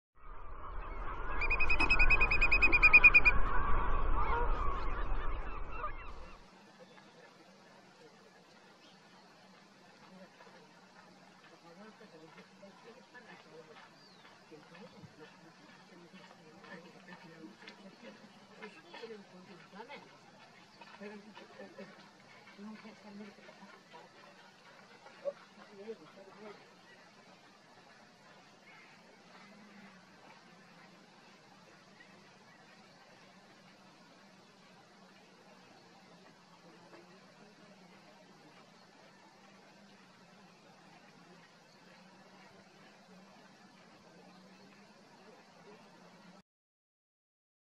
Estápulas en la brétema
Un grupo de 11 espátulas acompañadas de 2 garcetas comunes se alimentan juntas entre el barro del arenal de A Armona, cerca de la desembocadura del Miño. Estas grandes agregaciones son más habituales en esta época, cuando las grandes zancudas se desplazan entre sus zonas de reproducción y sus áreas de invernada.